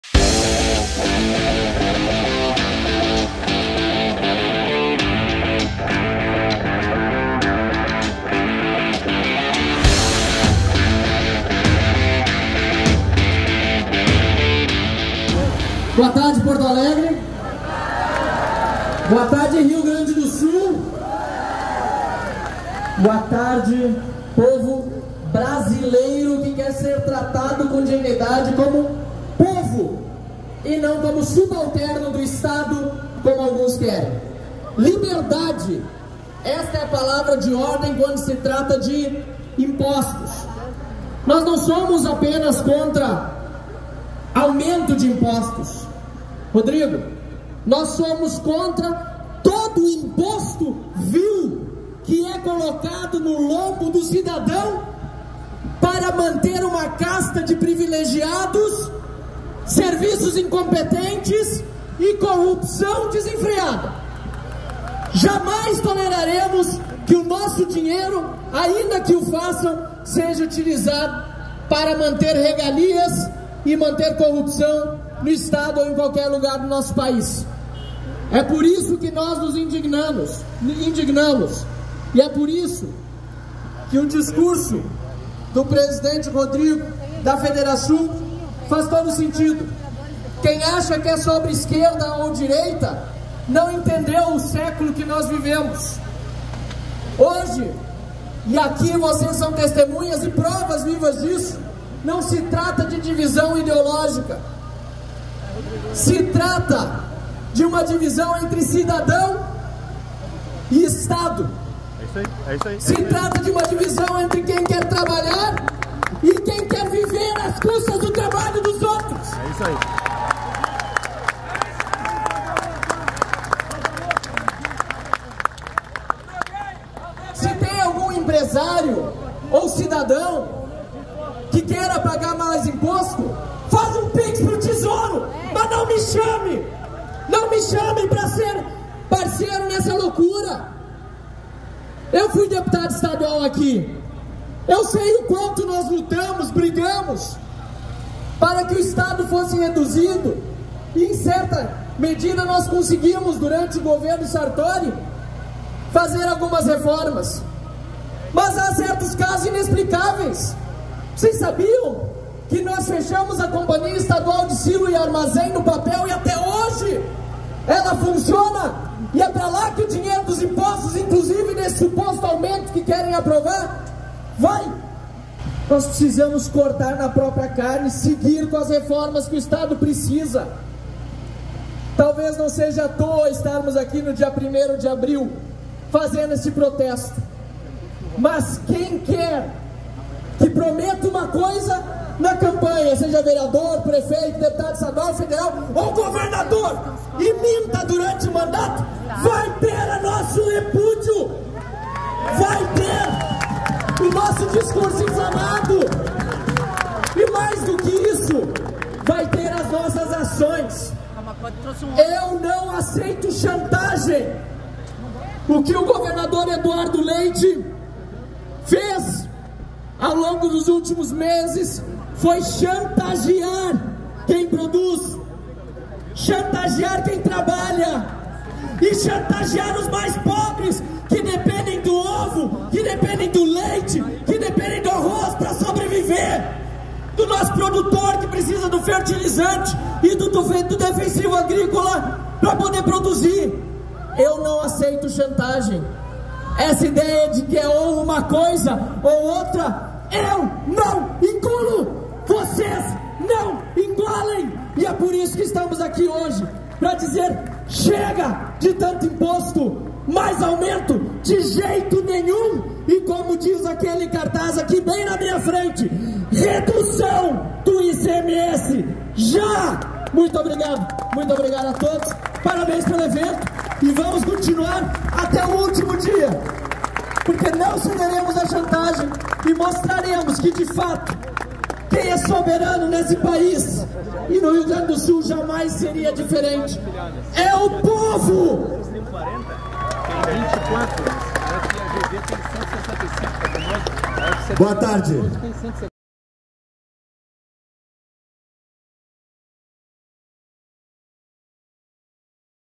O POVO GAÚCHO CLAMA: NÃO AO AUMENTO DE IMPOSTOS DE EDUARDO LEITE!
O pagador de impostos do Rio Grande do Sul já paga uma conta altíssima para financiar um Estado que entrega serviços muito ruins, empresas desnecessárias e até a corrupção. É vergonhoso que o governador ainda queira elevar o ICMS sobre itens da cesta básica para aumentar a arrecadação quando deveria estar cortando gastos!

Parabéns a todos que estiveram em frente ao Palácio Piratini nesta segunda-feira, 1o de abril. Não podemos permitir que o governador descumpra mais uma vez o que prometeu durante a campanha eleitoral e passe a conta de um estado inchado a quem produz!